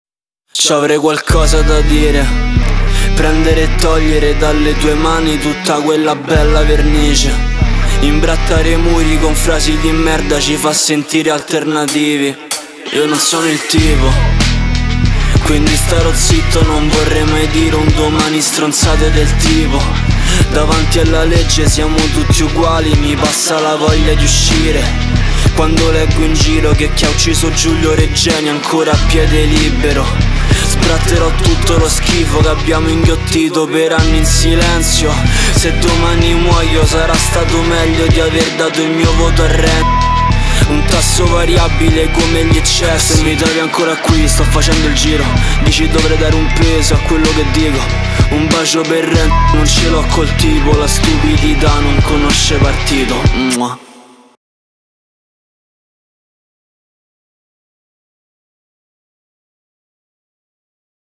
Mandark, 20 anni e un po' di cose da dire 📢🧠
"C'AVREI QUALCOSA DA DIRE" è un contenuto scritto e condiviso dall'artista sul suo profilo Instagram (@mandarkposer). E tu, c'hai qualcosa da dire?